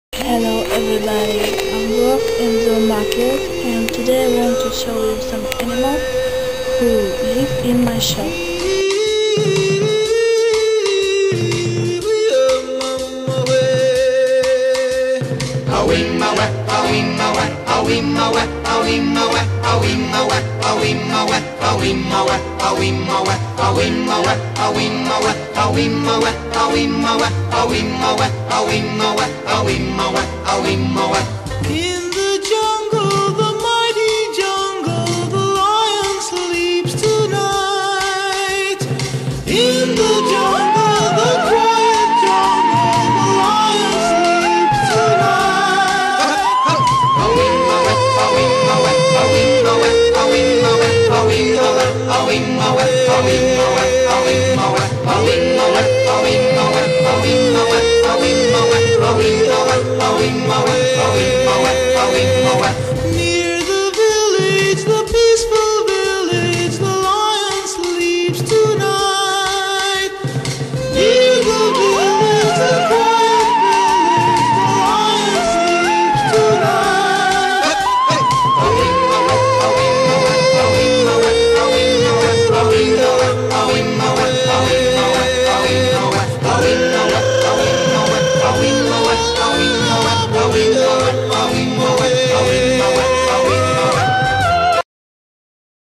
Work Jungle =))) ^_^
my favourite pets is  water turtles, tarantula, and rats. At various times there were many different animals, but someone will go live with new owners, but someone has grown and remained live in the shop. For example a large number of hamsters of various kinds. For me, all animals in a fun, and I am pleased work with them all day. Recently, I am very fond exotic animals and dream to buy a snake. but i so much love all rodents, and i can not feed the snake little rats and hamsters.